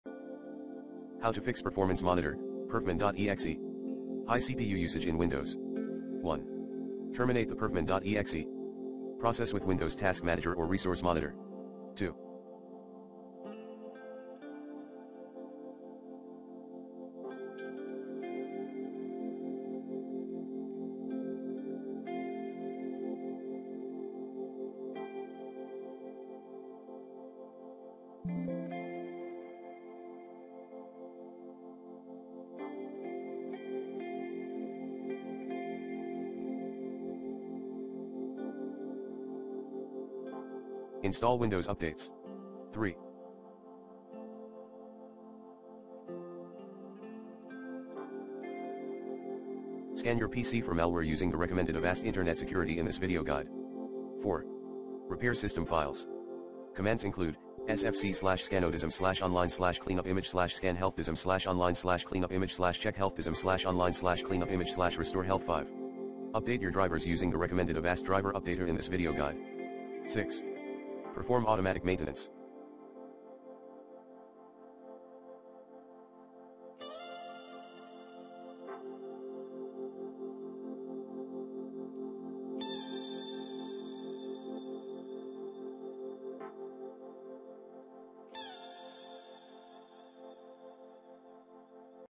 Fix Performance Monitor (perfmon.exe) high CPU usage in Windows
Here's how to Performance Monitor (perfmon.exe) high CPU usage in Windows. Run Avast Internet Security@ to clear potential malware. And Run Avast Driver Updater@ to keep your device drivers up-to-date, easily & effectively.

There are a few reasons why Performance Monitor (perfmon.exe) might be using high CPU usage on your Windows computer.

* **Corrupted or outdated Windows system files:** Corrupted or outdated Windows system files can cause Performance Monitor to use high CPU usage. To fix this, you can try running the System File Checker (SFC) scan.
* **Malware infection:** A malware infection can also cause Performance Monitor to use high CPU usage. To scan your computer for malware, you can use a reputable antivirus program.
* **Background tasks:** Background tasks, such as antivirus scans or file downloads, can also cause Performance Monitor to use high CPU usage. To stop these tasks, you can close them from the Task Manager.
* **Hardware problems:** In some cases, hardware problems, such as a faulty hard drive or memory module, can also cause Performance Monitor to use high CPU usage. If you have recently installed new hardware, you can try uninstalling it to see if that fixes the problem.

If you are still having problems after trying these steps, you can contact Microsoft support for help.

Here are some additional things you can do to troubleshoot high CPU usage caused by perfmon.exe:

* **Check for updates:** Make sure that your computer is up to date with the latest Windows updates.
* **Disable unnecessary services:** Some services can run in the background and use up CPU resources. You can disable these services to free up CPU usage.
* **Use a task manager:** A task manager can show you which processes are using up CPU resources. You can use this information to identify the process that is causing the problem and take steps to fix it.
* **Run a virus scan:** A virus scan can detect and remove malware that may be causing the problem.
* **Restore your computer to a previous point in time:** If you have created a restore point, you can restore your computer to a time when the problem was not occurring.

If you are still having problems after trying these steps, you may need to contact a computer technician for help.